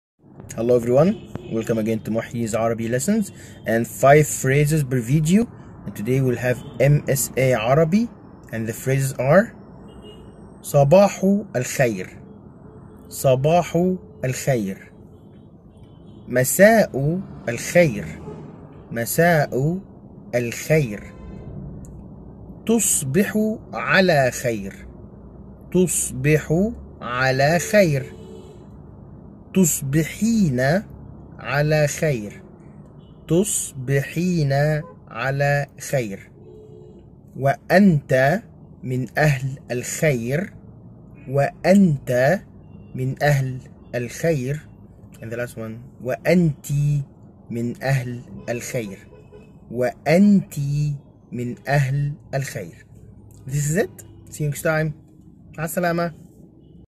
MSA Arabic Greetings Lesson 2
5 MSA Arabic expressions per video
today we will learn
صباح الخير good morning
مساء الخير good evening
تصبح على خير good night for a man (may you wake up on goodness)
تصبحين على خير good night for a woman (may you wake up on goodness)
و انت من اهل الخير good night back for a man ( and i wish you are a good person in the morning)
و انتِ من اهل الخير  good night back for a Woman ( and i wish you are a good person in the morning)
In this Video we will learn 5 Expressions  in MSA Arabic about Greetings
follow and  repeat